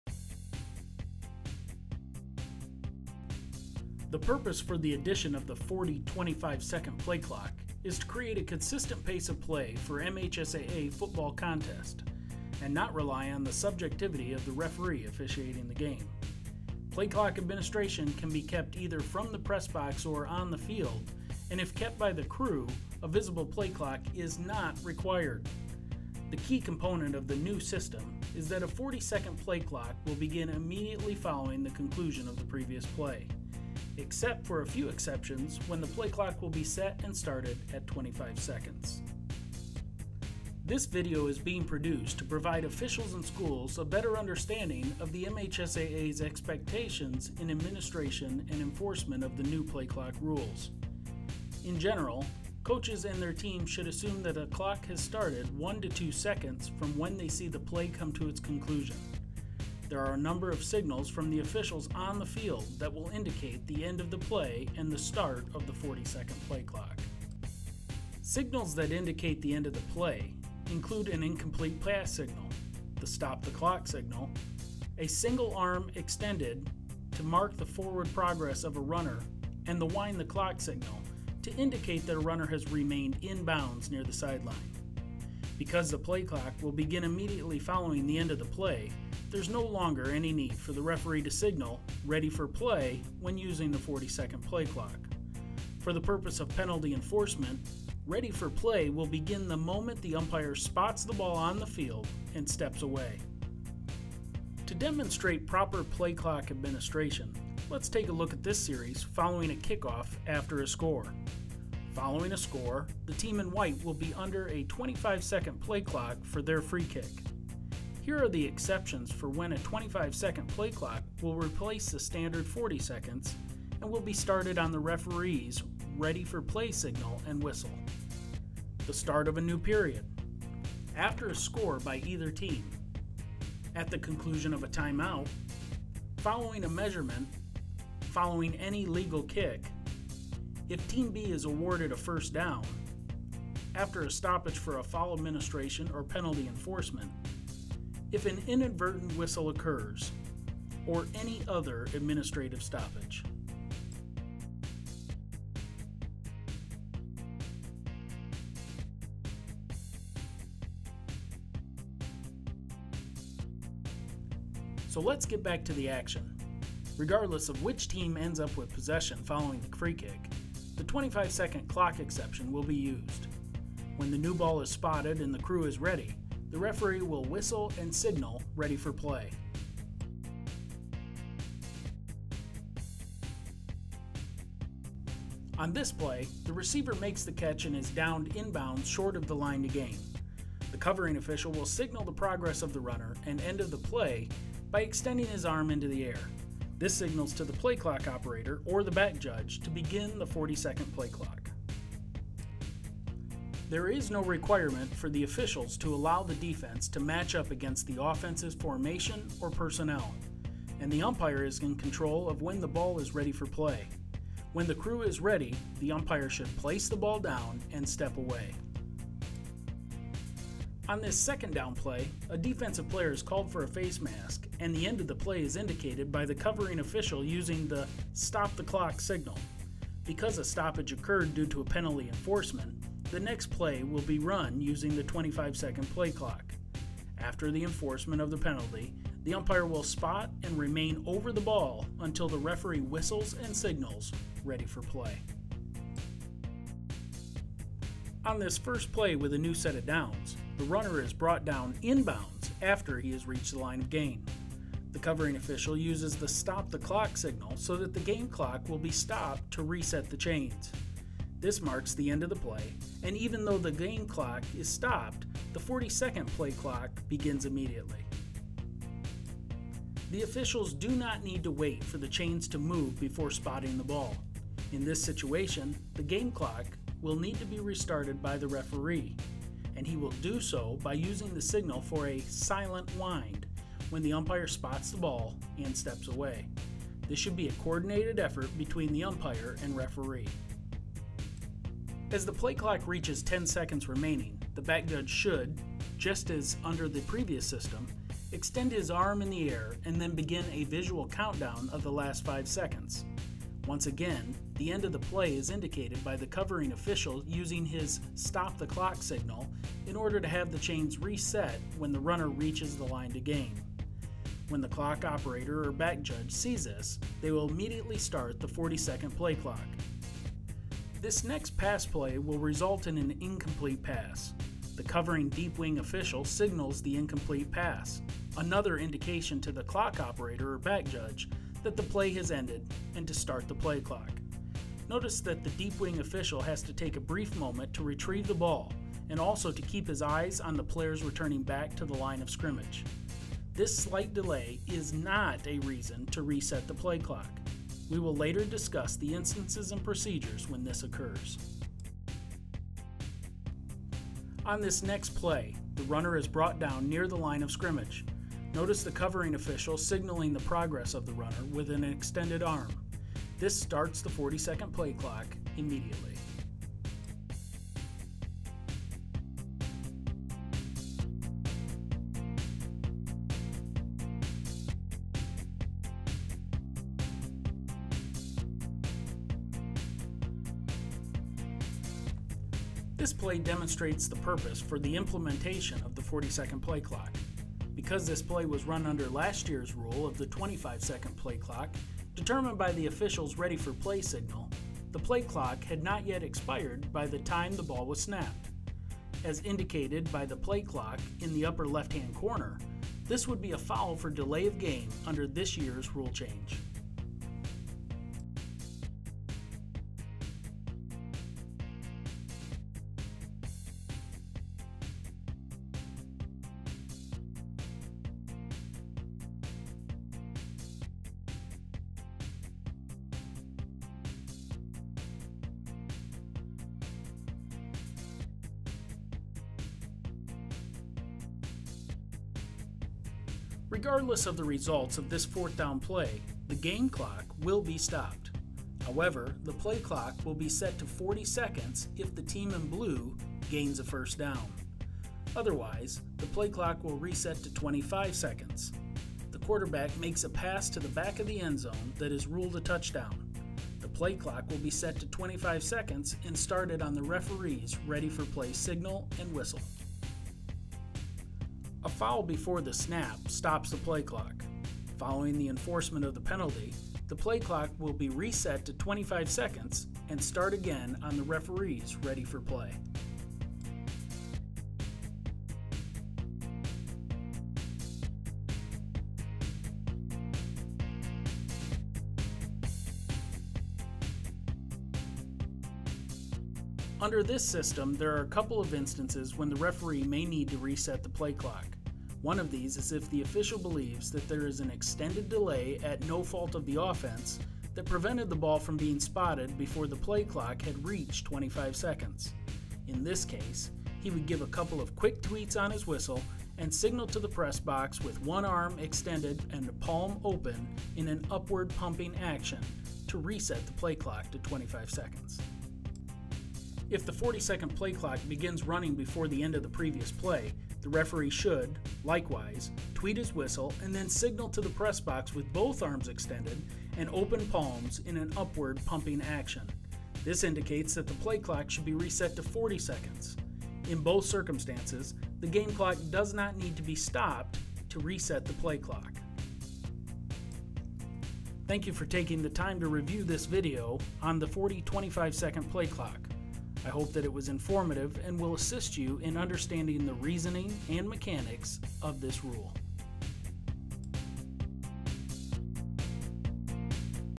MHSAA Officials Program: Football 40-Second Play Clock (2019)
This video on the 40/25 –Second Clock is to provide schools, coaches and officials a resource for the protocol, mechanics and implementation of the 2019 NFHS rule change in football.